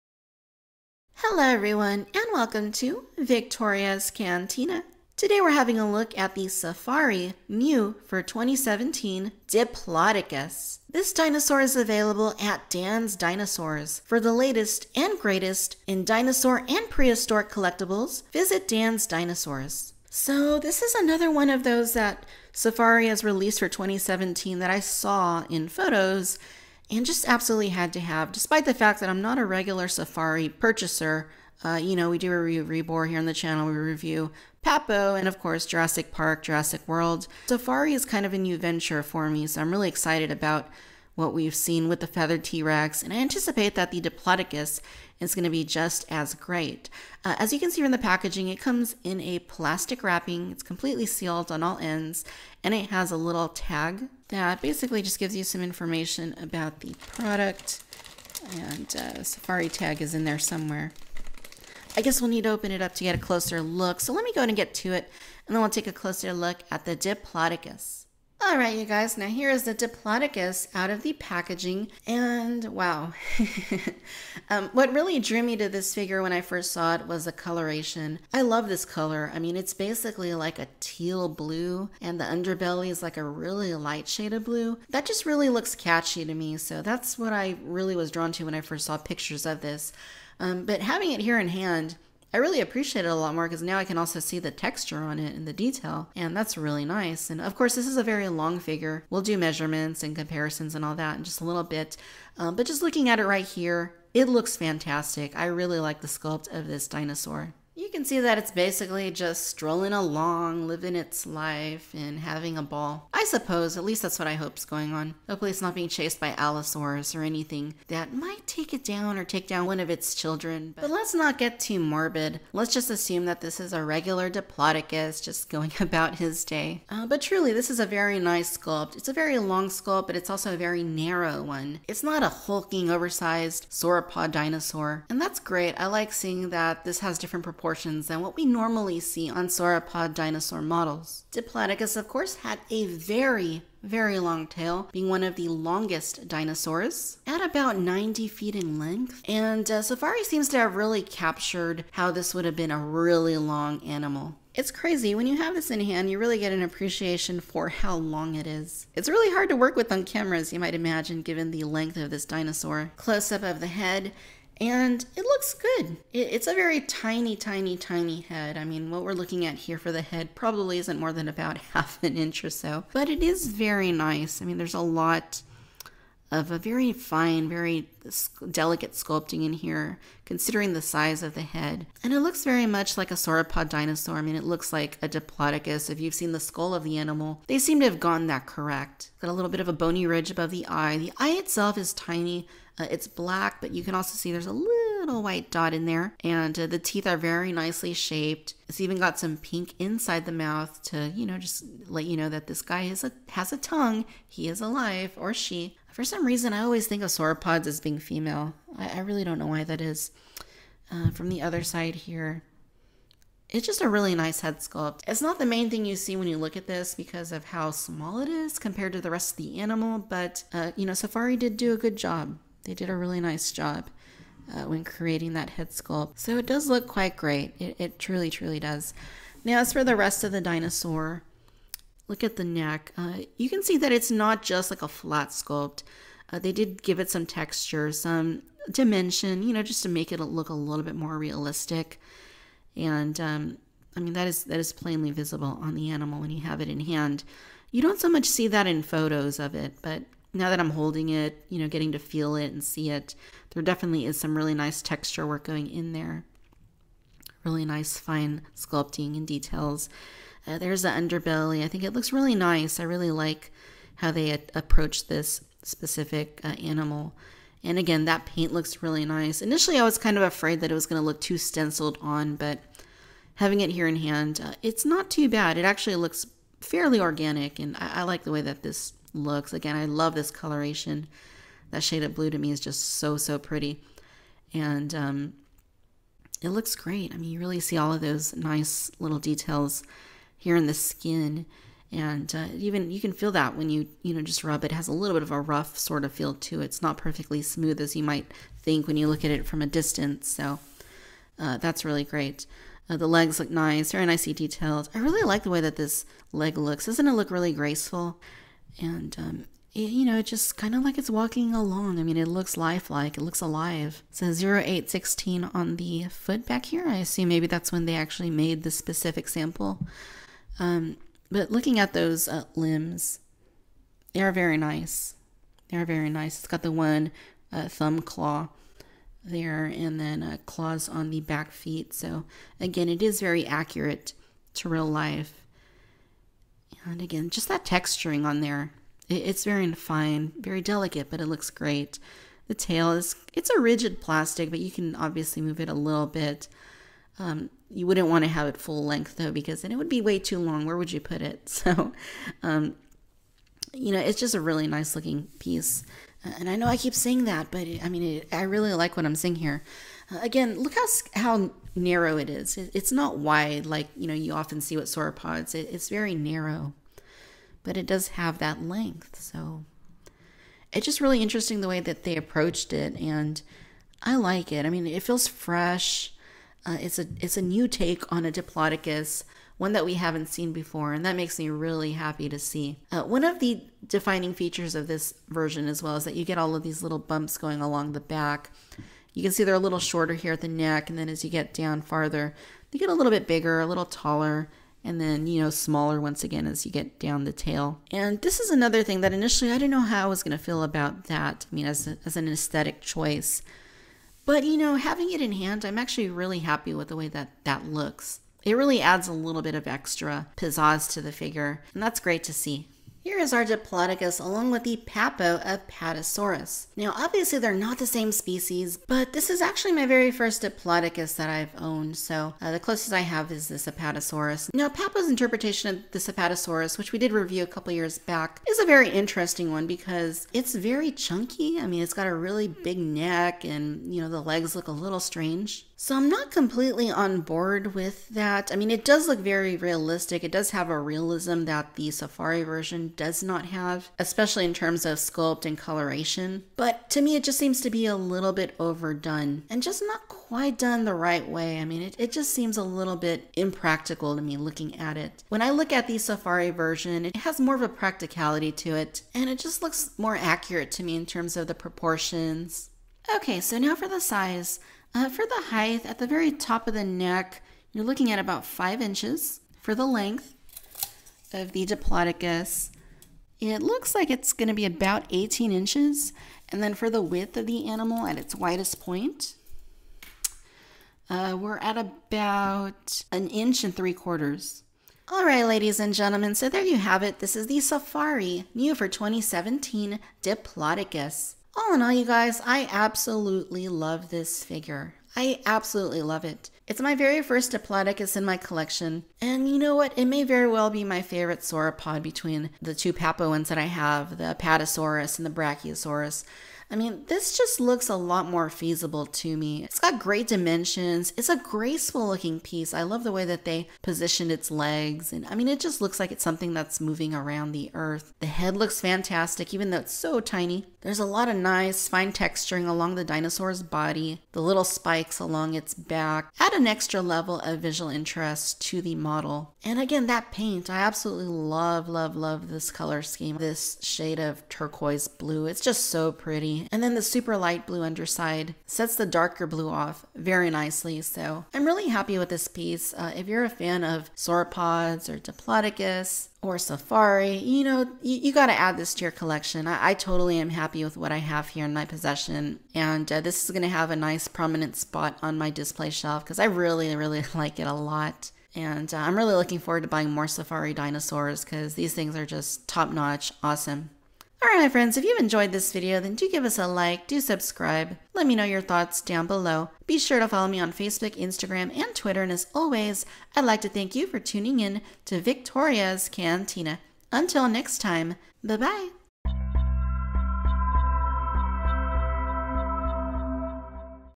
Safari Ltd. 2017 Diplodocus Review
In this Victoria's Cantina review, we take a look at the Safari Ltd. Diplodocus! New for 2017.

This video is the copyrighted property of Victoria's Cantina Productions.